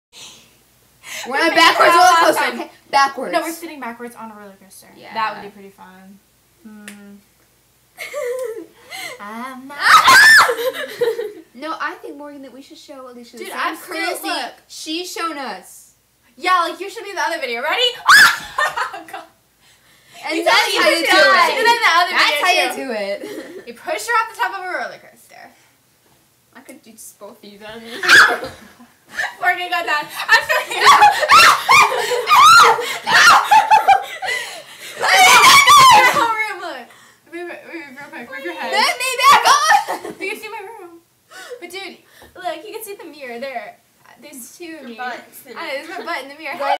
Rollercoaster: 1 year later!!
the same rollercoaster, the same people [with the addition of morgan!!] and the same fun! it was so much fun esp. to see how we've all changed ^_^